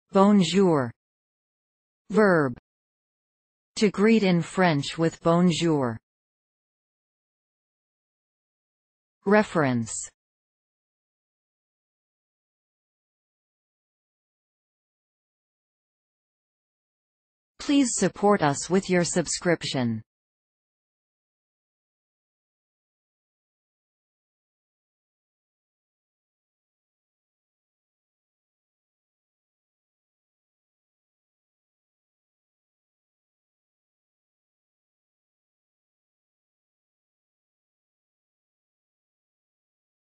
Bonjour | Definition of bonjour 📖 📖 📖 📖
See here, the definitions of the word bonjour, as video and text.

bonjour (verb)
    To greet in French with "bonjour".

Reference:
    bonjour